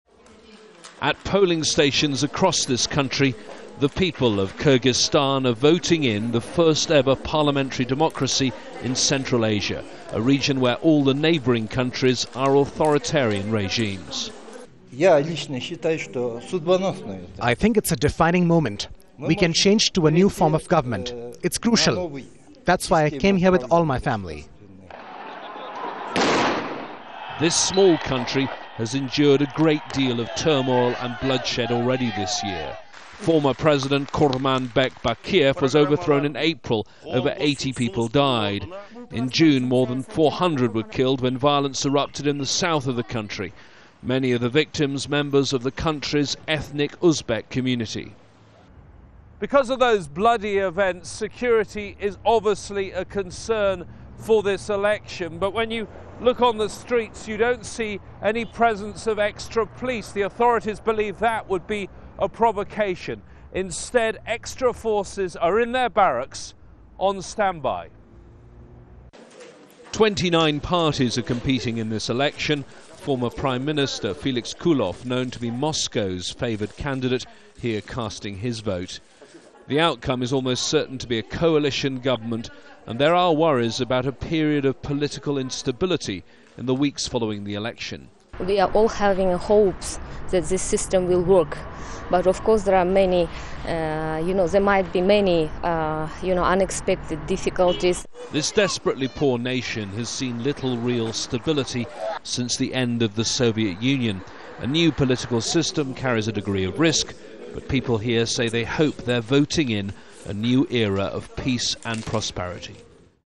Kyrgyz vote for first democratically elected parliament
The people of Kyrgyzstan are voting in an historic election, which will create the first parliamentary republic in central Asia.

The country's interim leader hopes it will put an end to months of ethnic tension that followed the overthrow of the government in March.

Al Jazeera's James Bays has more from Bishkek, the capital.